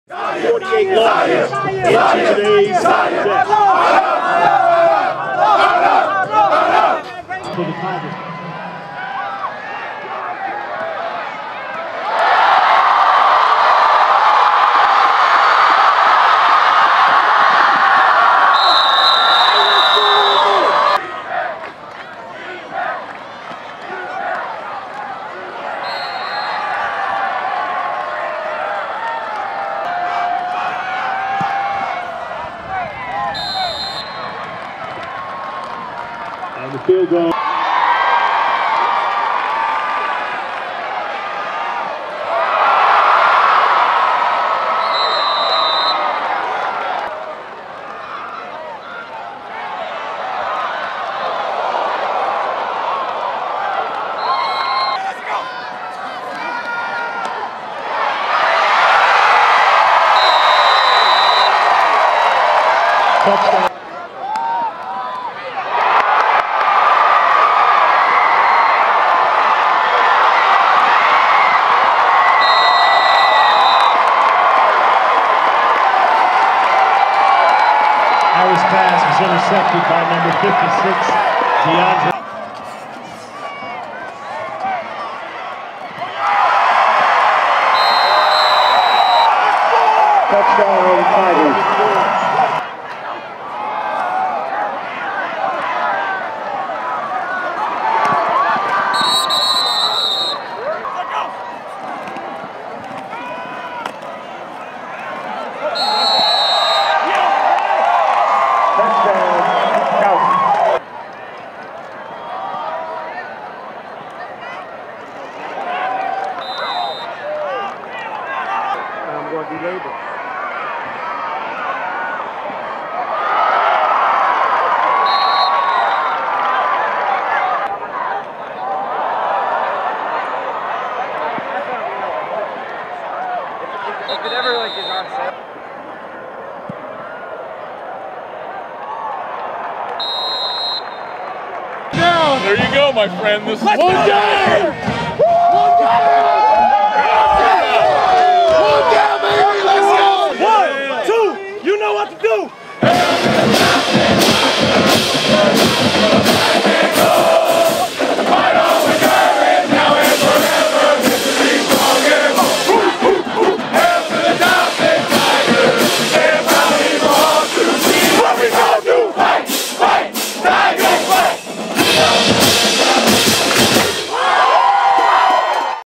Towson Football wins the Battle for Greater Baltimore 36-10 over Morgan State
The Towson University Tigers claimed a decisive victory of the Morgan State Bears, 36-10.